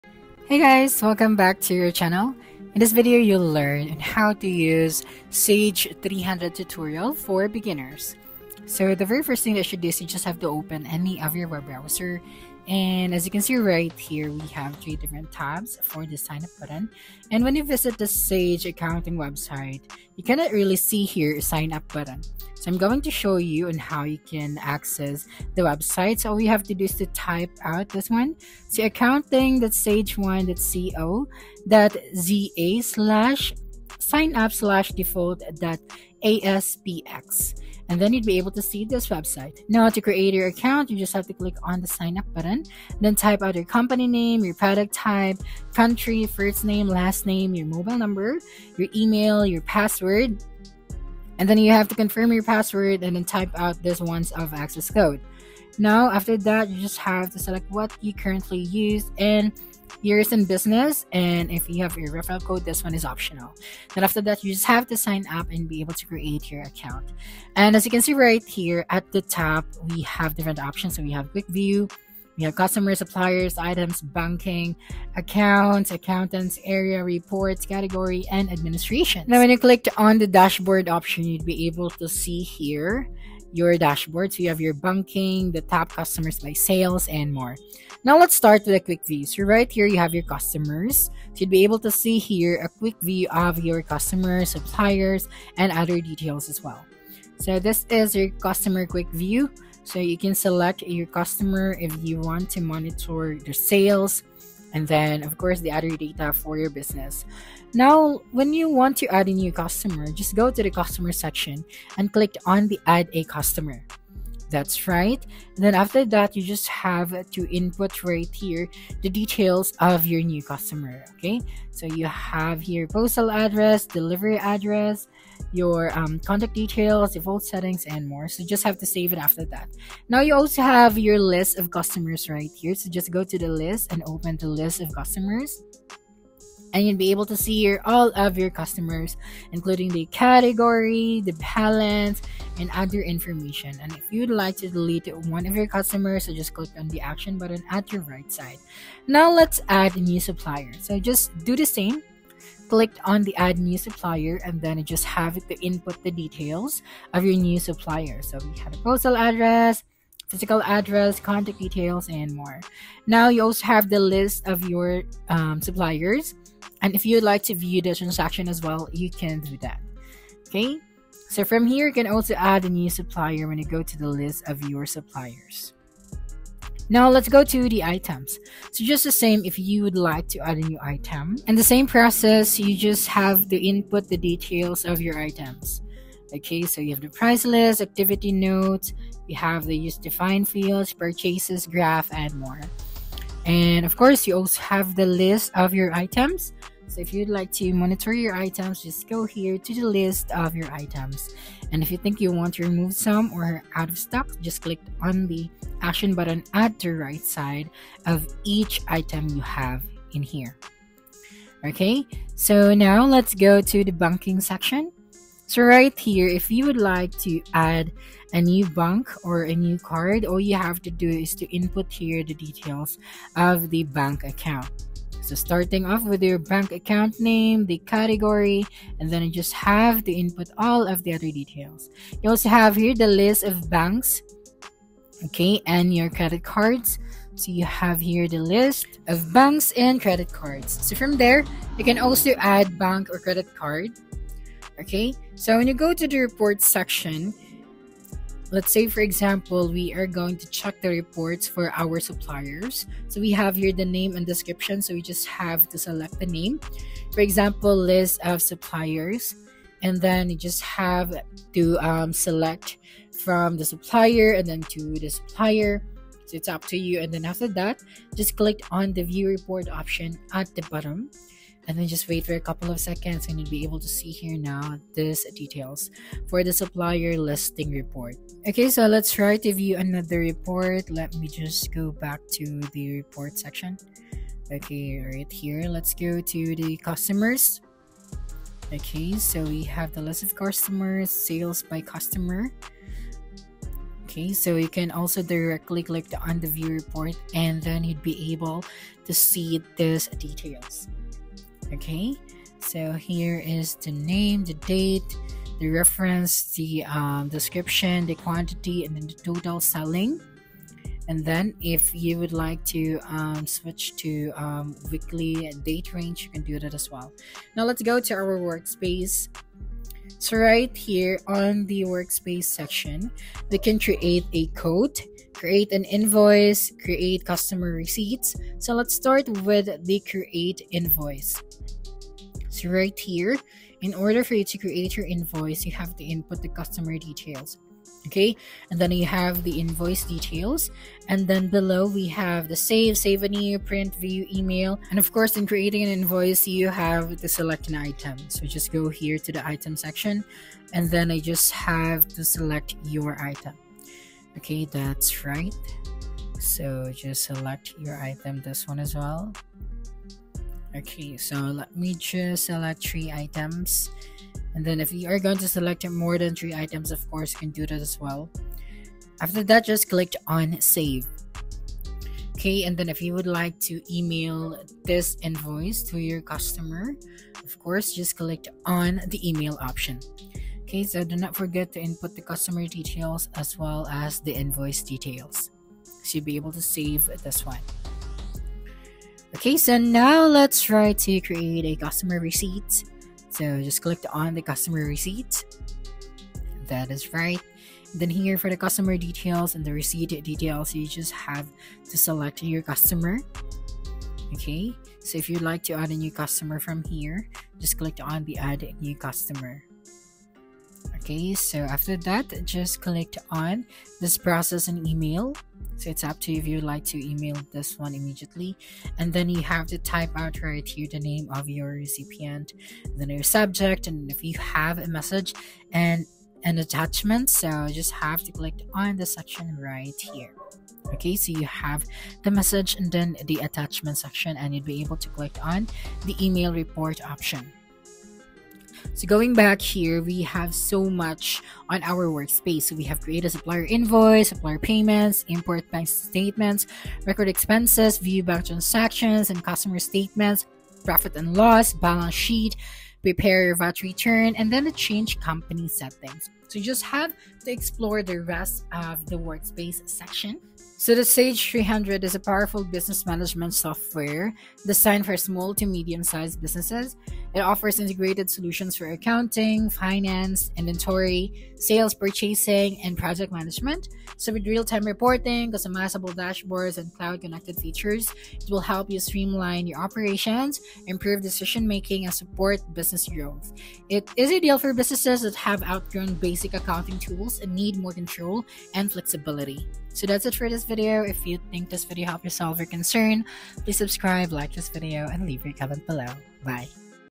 Sage 300 Tutorial for Beginners || Sage 300 Demo Training 2025 (Full Guide)
Learn the ins and outs of Sage 300 with this beginner tutorial! This demo training will guide you through all the essentials you need to know for 2025.